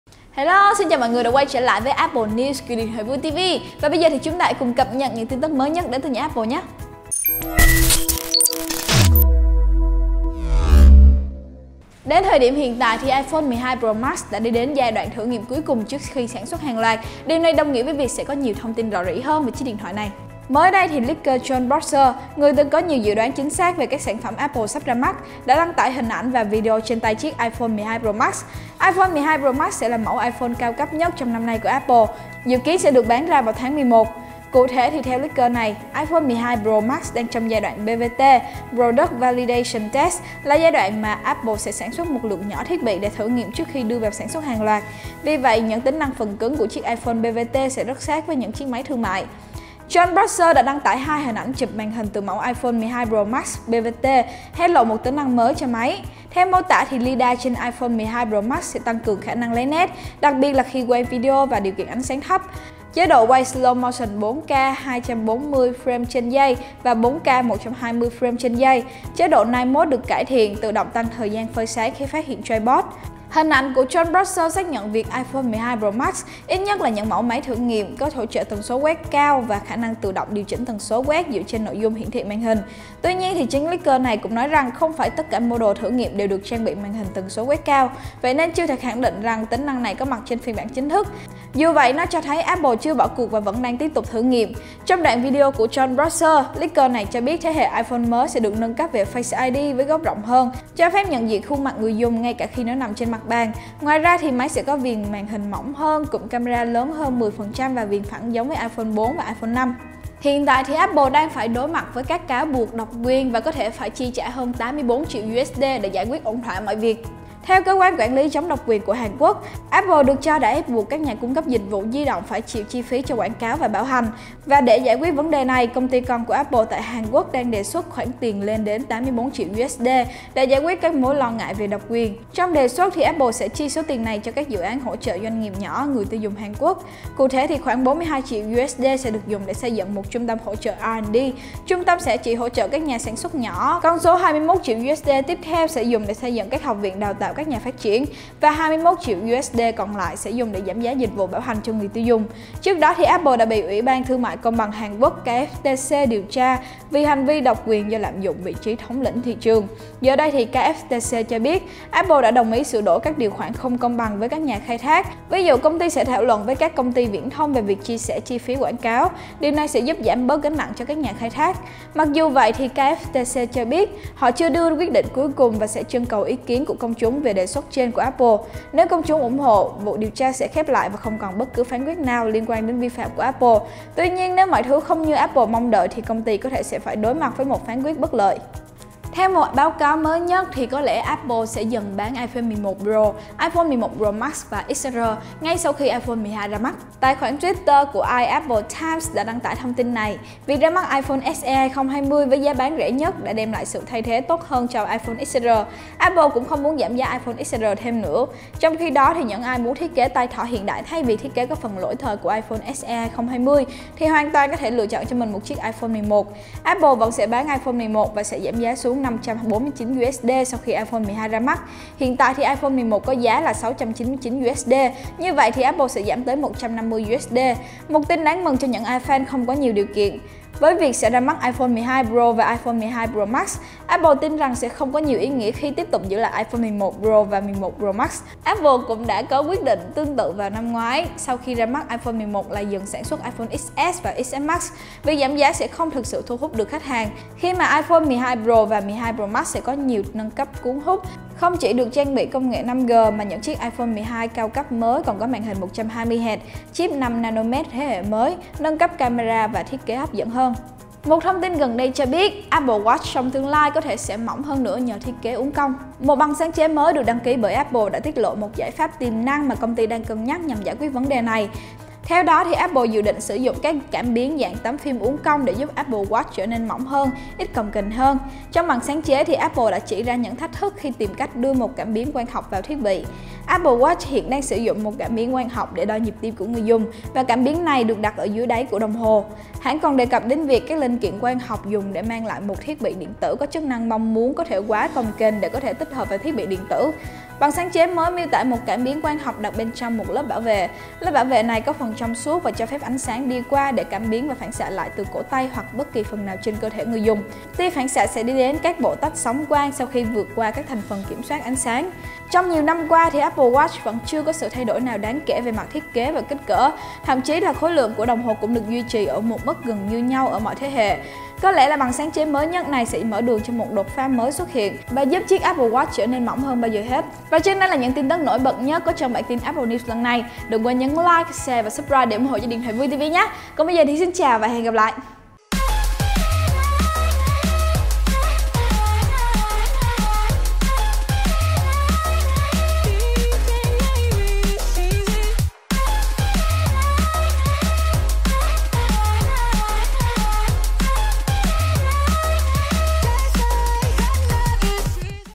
Apple News 68: Rò rỉ clip iPhone 12 Pro Max, Appl sẽ khai Tử iPhone 11 Series | Điện Thoại Vui TV
"Apple News số 68 sẽ cập nhật thông tin sau:
- iPhone 12 Pro Max bị lộ clip thông số
- Apple có thể bị kiện tại Hàn Quốc
- iPhone 11 Series sẽ có thể dừng bán khi iPhone 12 series ra mắt
- Apple Watch sẽ trở nên mỏng hơn nữa trong tương lai
📱 Điện thoại - Phụ kiện GIÁ TỐT NHẤT - CHÍNH HÃNG tại CellphoneS